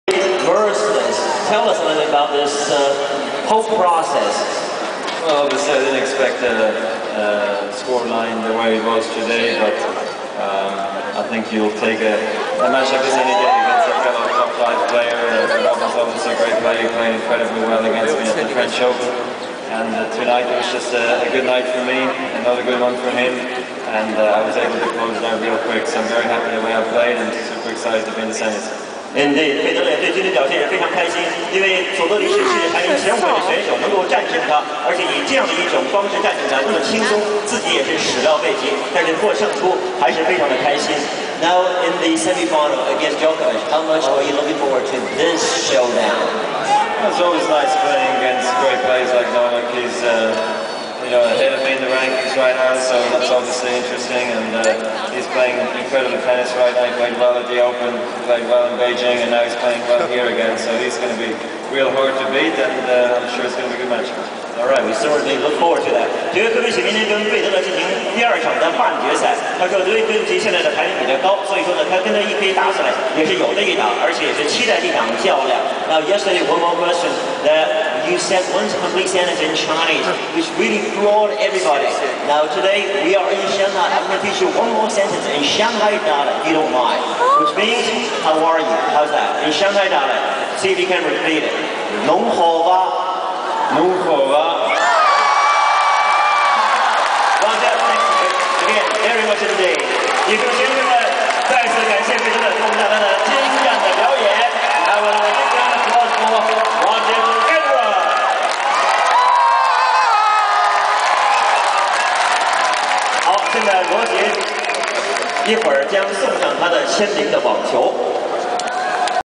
Roger Federer said Shanghai Dialect "侬好伐" (How are you?)
After defeating Robin Soderling to reach the semi-final of Shanghai Rolex Masters, Roger Federer was invited to show the fans some of his linguistic talents. Here Roger said How are you in Shanghai dialect!